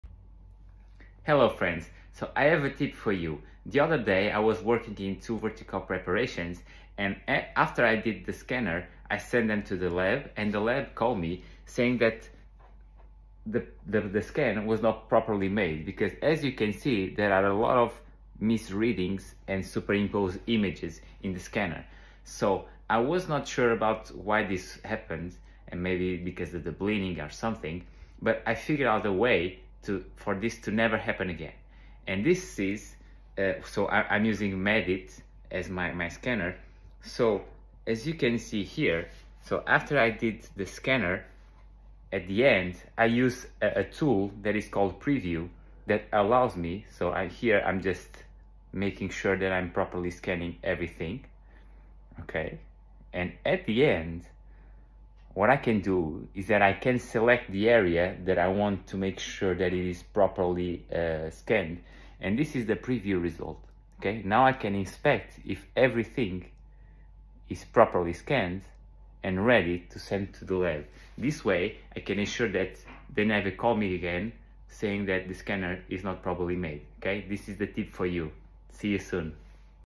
DIGITAL DENTISTRY TIP #1 - MEDIT RESULT PREVIEW
DIGITAL DENTISTRY TIP

🕵️‍♂️ It is very important to check your scans before you send them to the lab. Sometimes they look ok during the scanning, but after the post processing we can see some errors.

🧠 In @meditcompany intraoral scanner you have a feature called “result preview” that allows you to do the post processing of a specific area and check if your preps are properly scanned.